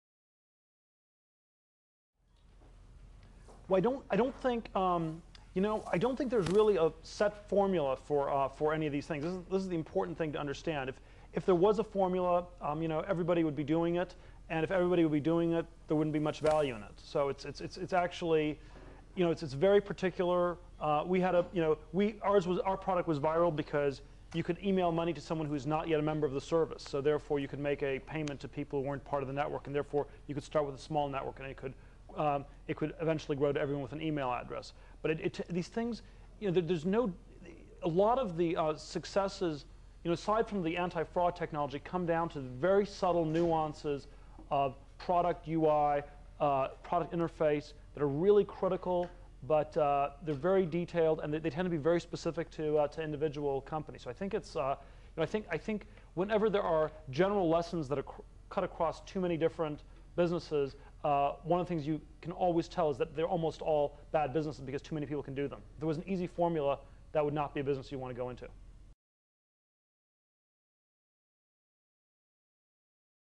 Peter Thiel-Viral Marketing
There is no set formula to make viral marketing successful. Rather, it depends on the situation. It worked with Paypal because customers were able to transfer money to non-customers, therefore growing the network
      quickly.